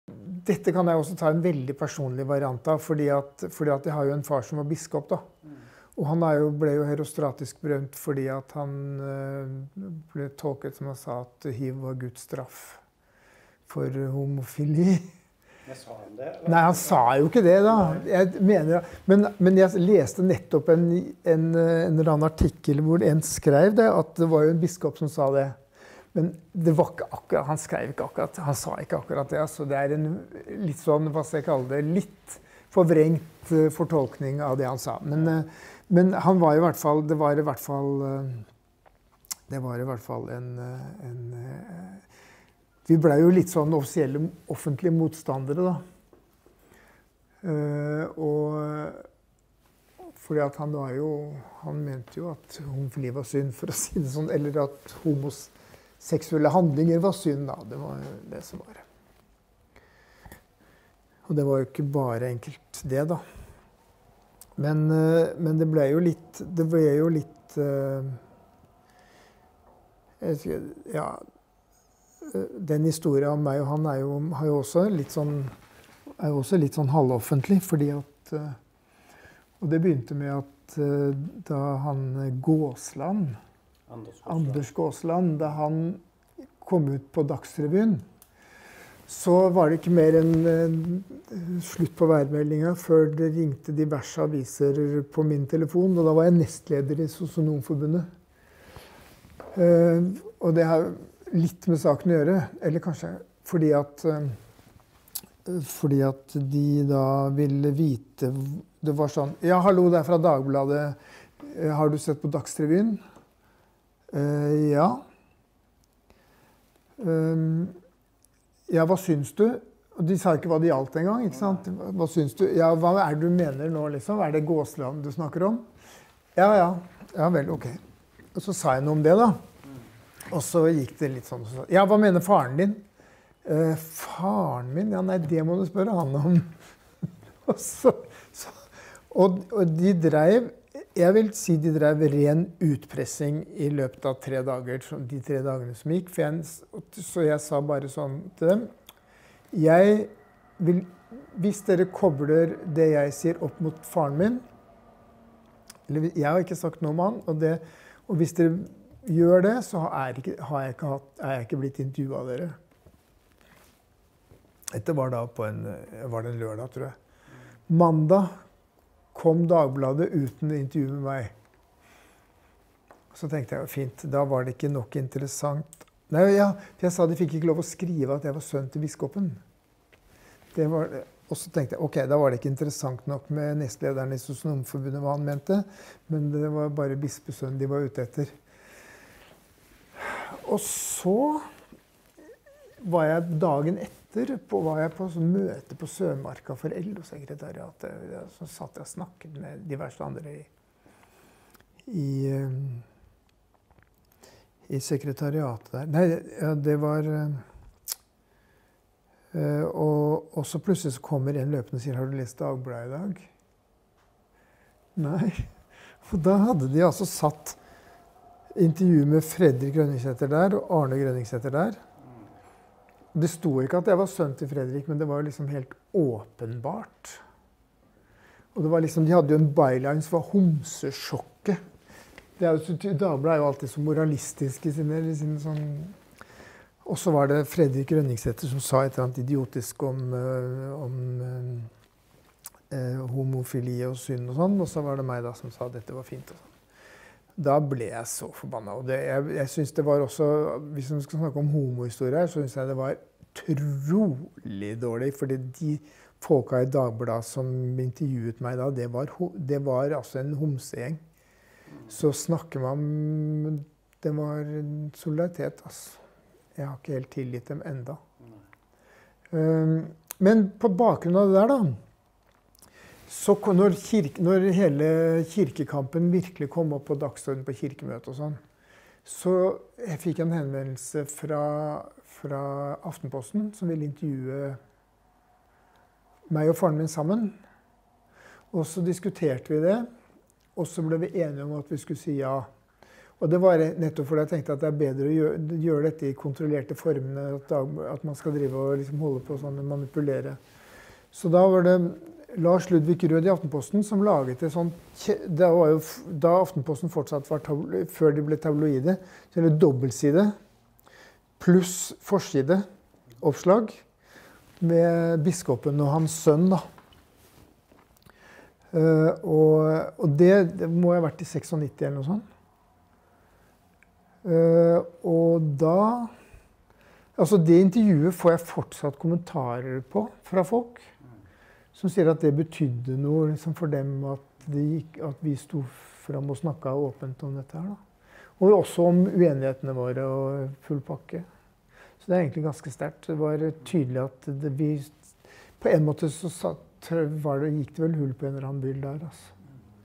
- Det var bare biskopens sønn de var ute etter.
Arne Grønningsæter følte seg presset av Dagbladet da han ikke ville stå fram som bispesønn.

Historien er en del av Skeivt arkivs livsminneprosjekt. Les mer